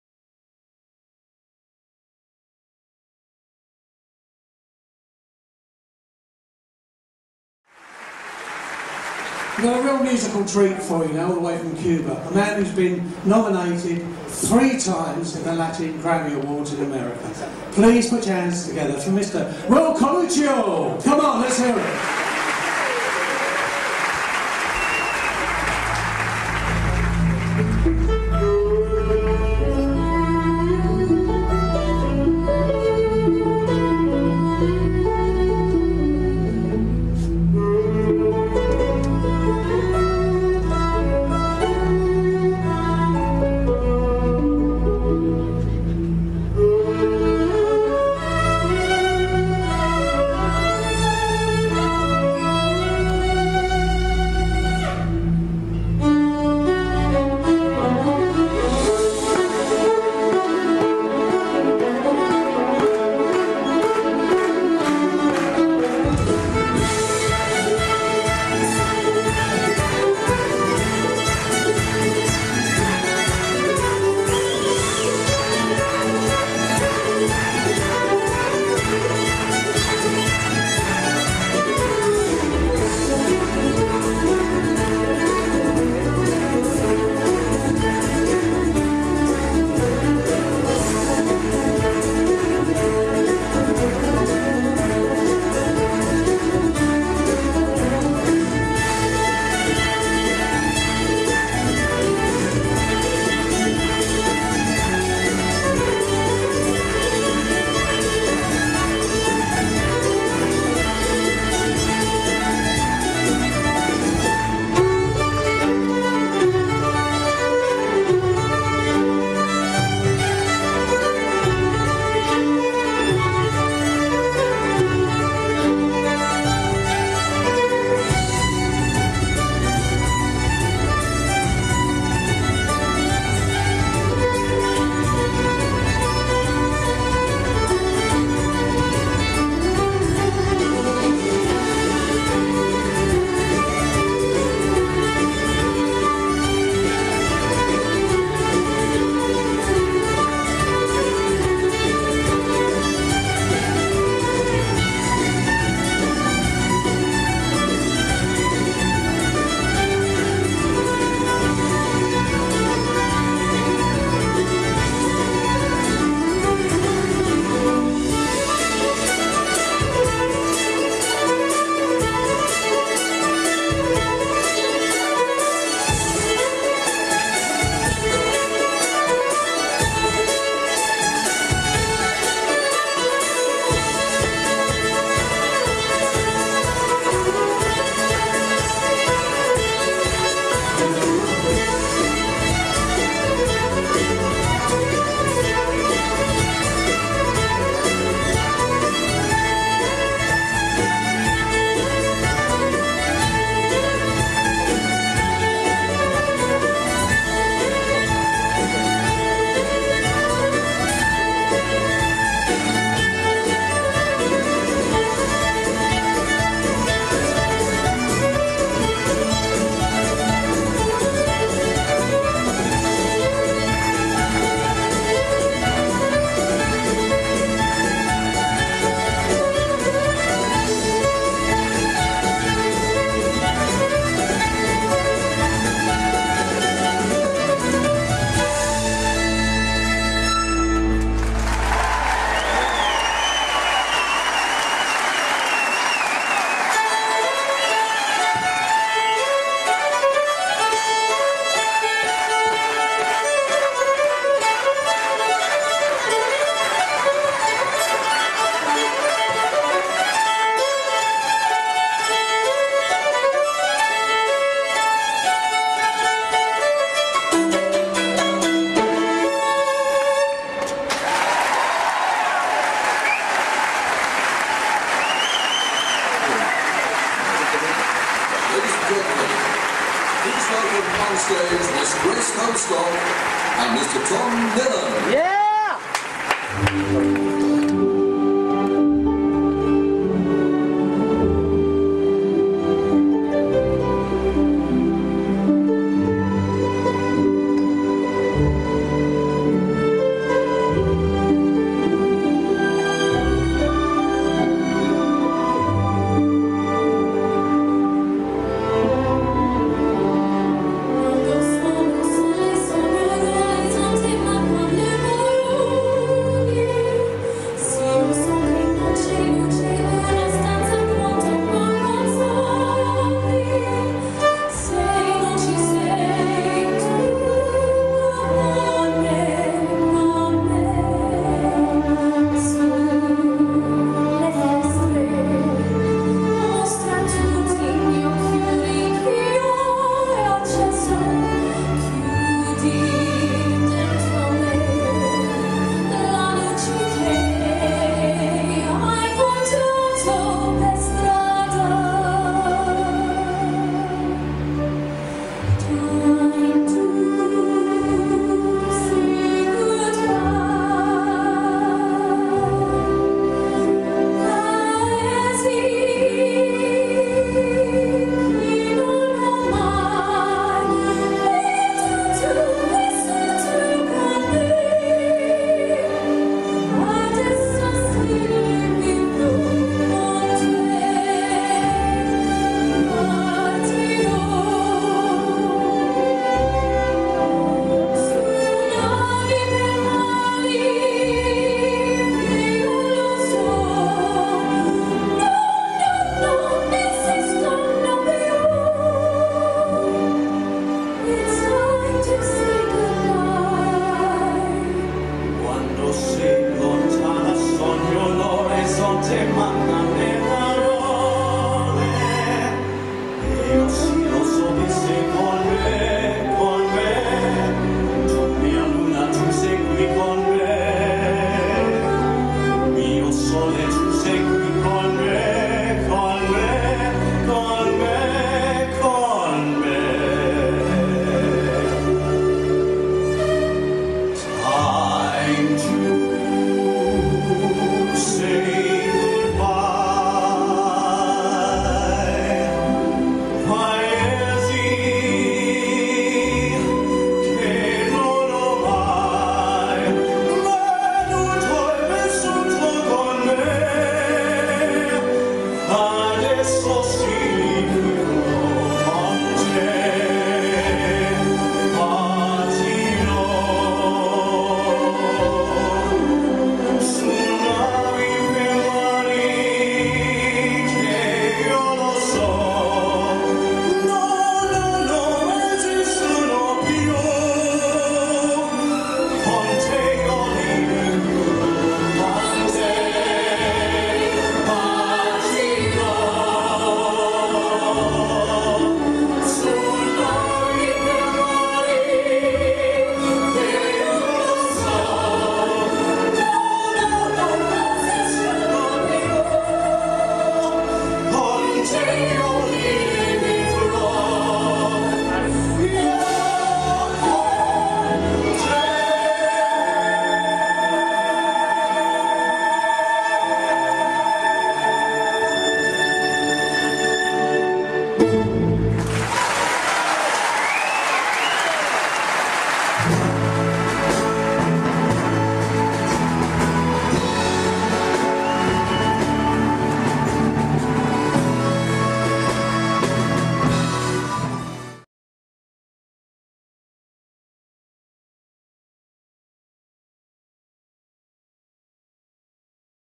WILL CORUJO AT LONDON PRINCE OF WALES THEATRE.m4v
WILL CORUJO EN EL TEATRO PRINCE OF WALES EN LONDRES
INVITADO ESPECIAL AL THE ROYAL MASONIC VARIETY SHOW 2008